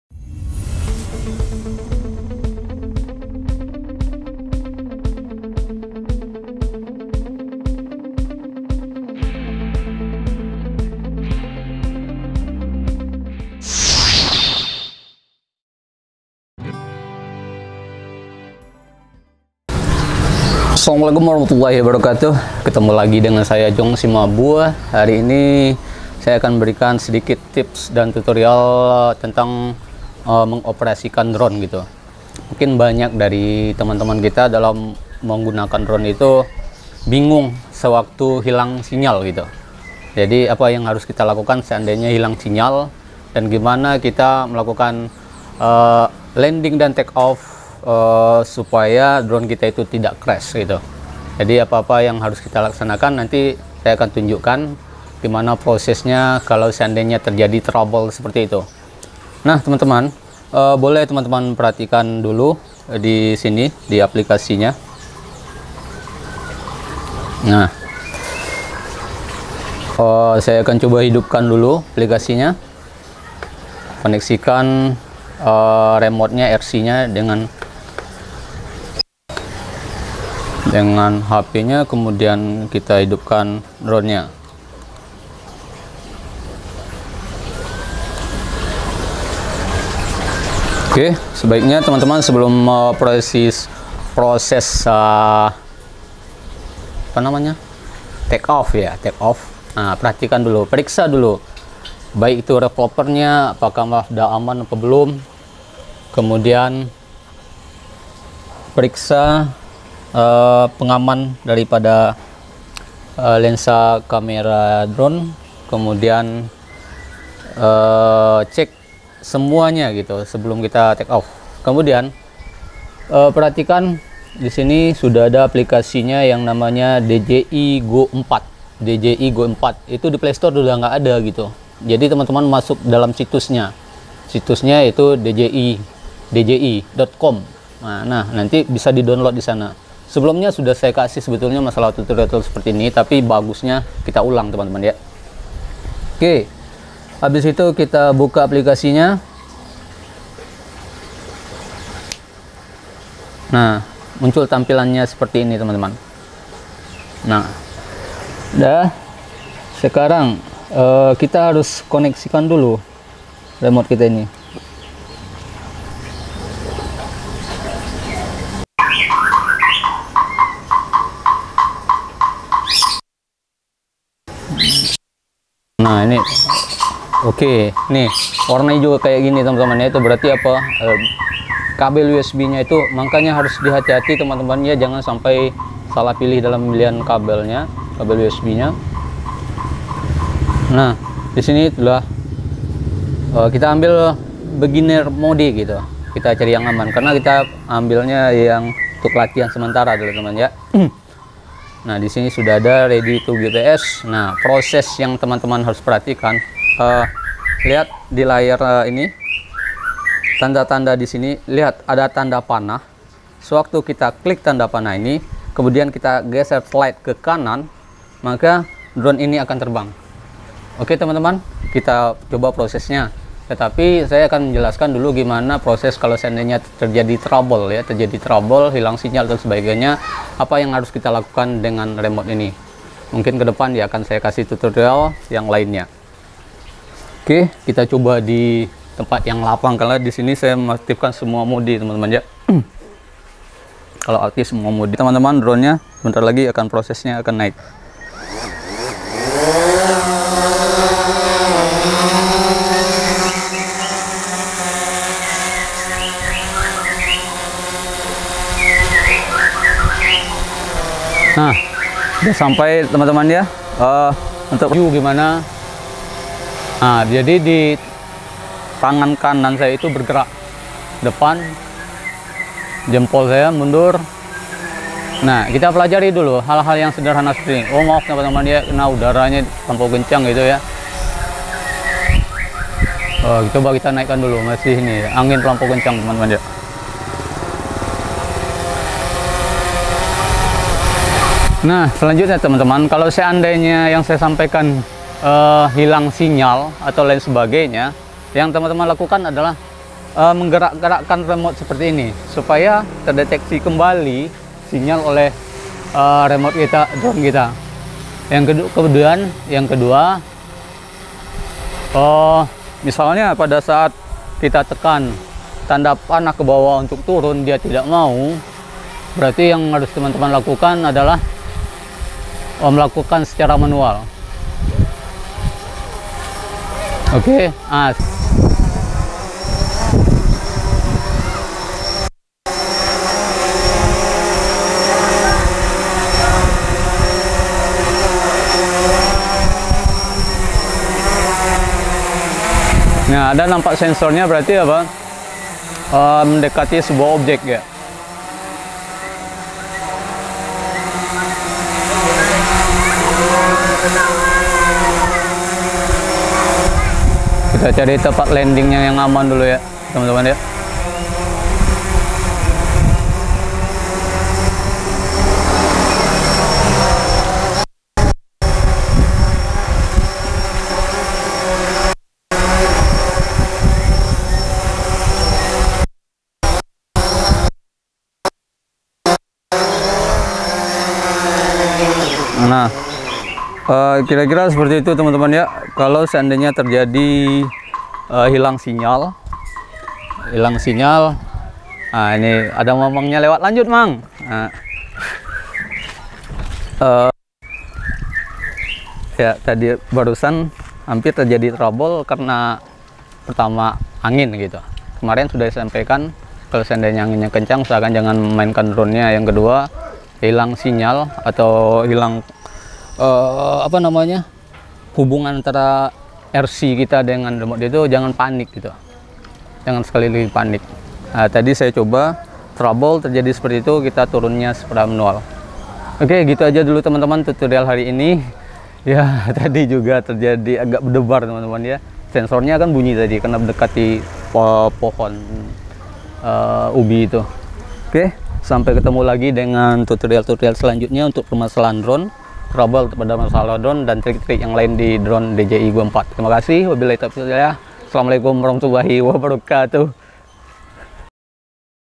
EMERGENCY LANDING DRONE DJI 4 DENGAN AMAN
Cara Emergency landing Drone manual dan aman.
adalah sebuah proses dengan mengutamakan RC dalam melakukan pendaratan darurat jika terjadi hilang sinyal atau tiadak berfungsinya tombol home pada remot.
proses ini dapat dilakukan jika terjadi troubel troubel pada monitor dan hilang sinyal.
ini ditandai dengan salah satunya gambar video patah-patah atau berwarna hijau.
kebanyakan pada massalah yang terjadi pada proses ditandai juga dengan bergeraknya dron tidak terkendali, kadang sudah hampir mau landing, dron naik sendiri.
mudah-mudahan video ini dapat membantu para pengguna drone.